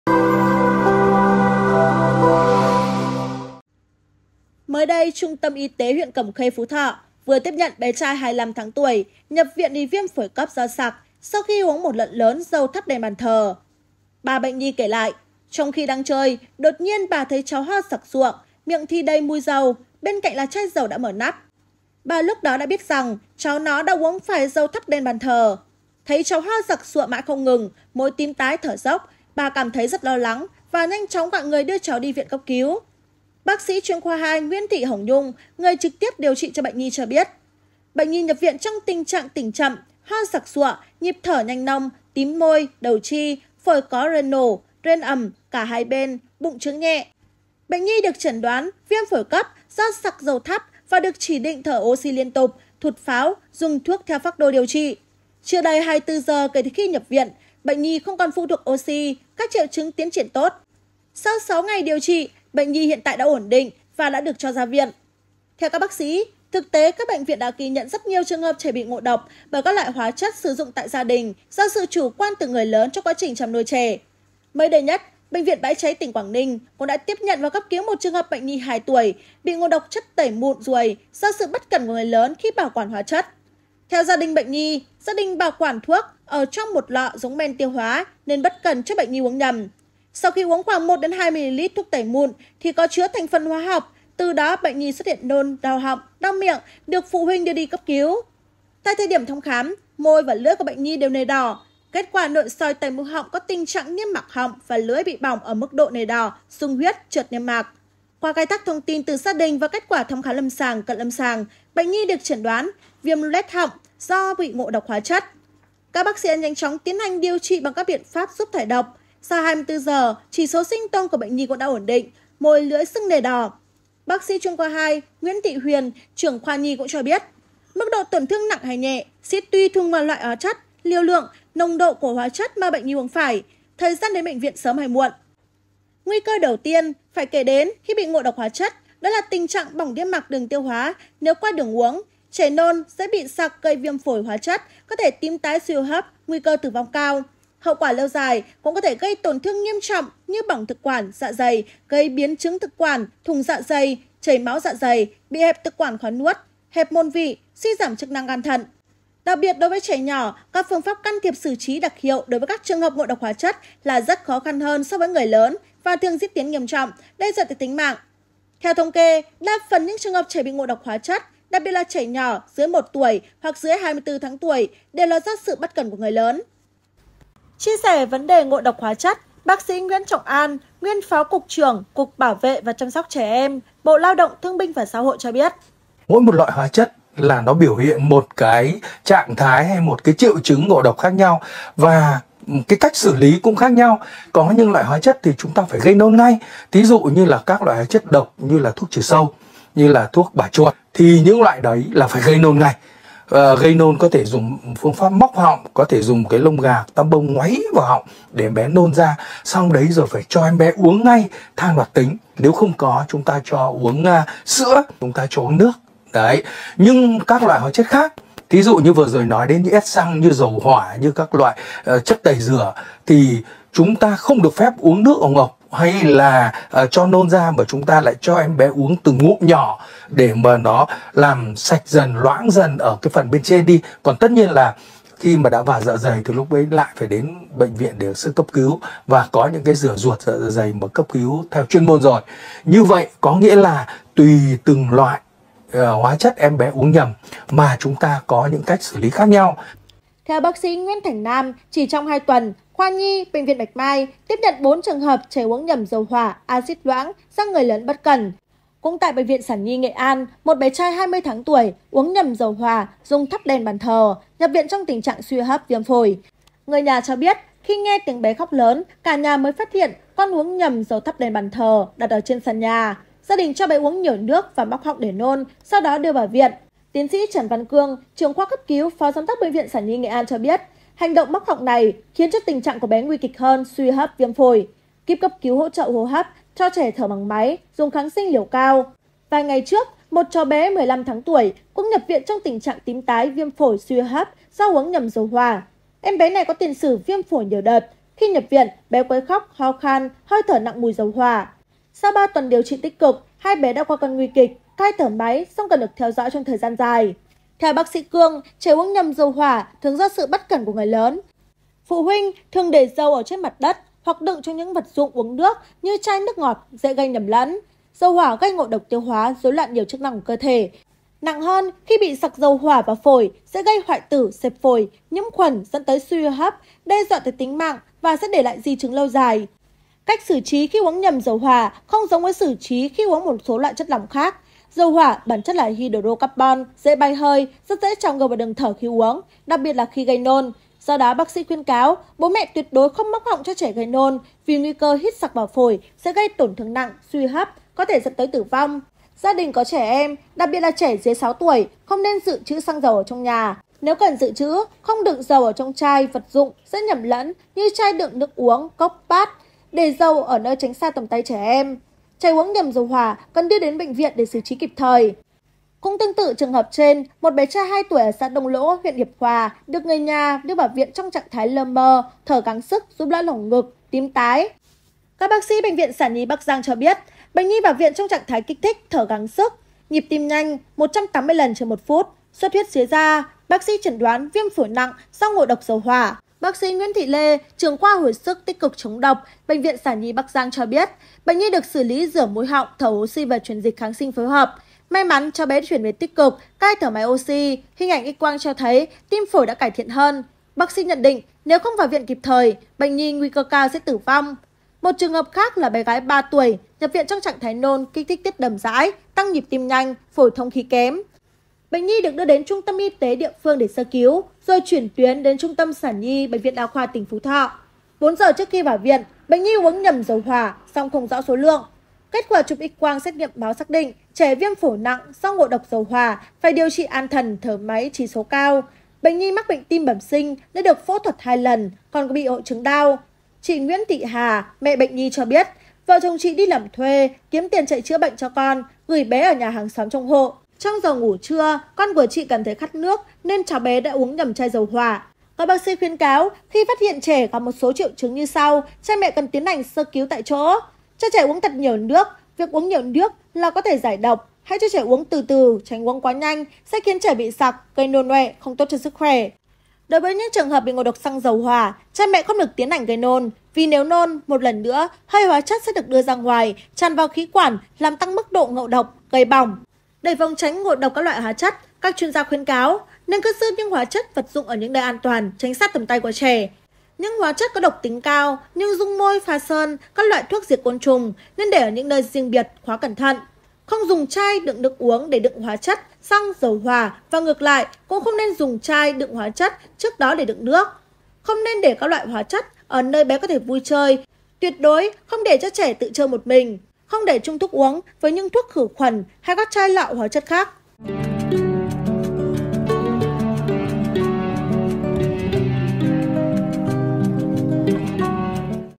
Uống Nhầm Dầu Thắp Đèn Bệnh Nhi 25 Tháng Tuổi Viêm Phổi Cấp | SKĐS
SKĐS | Trung tâm Y tế huyện Cẩm Khê (Phú Thọ) vừa tiếp nhận bé trai 25 tháng tuổi, nhập viện vì viêm phổi cấp do sặc, sau khi uống một lượng dầu thắp đèn thờ.

Mời quý vị và các bạn xem thêm: Thông Tin Về Hai Ca Sốc Phản Vệ Sau Tiêm Vắc Xin Ở Trung Tâm Tiêm Chủng Long Châu | SKĐS